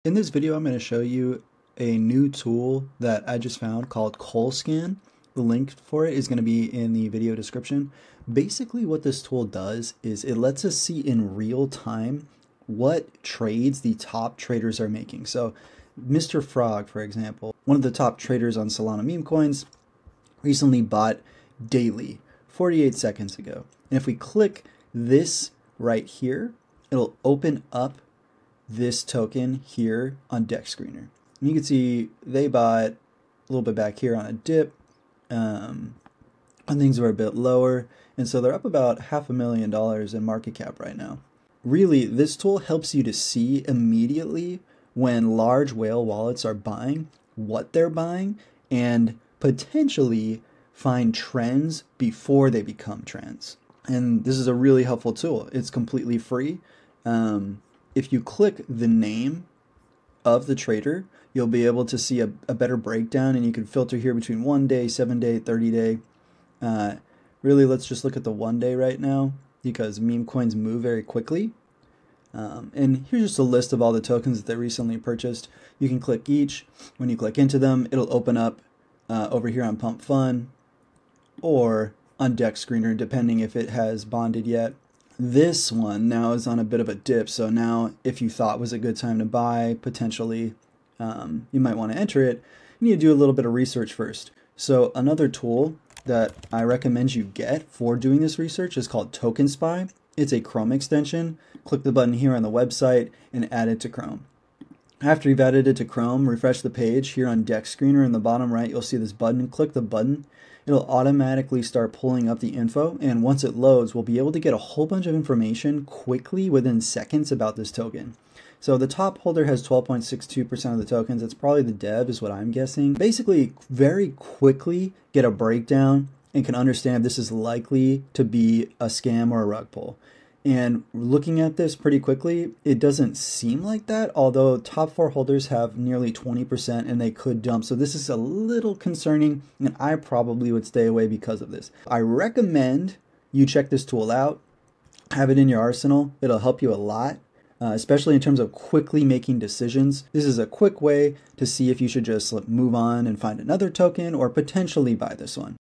Inside the Mind of a Crypto Whale: Tracking Top Wallets in Realtime with Kolscan
In this video I show how you can track top trading wallets on Solana in realtime using a few simple tools.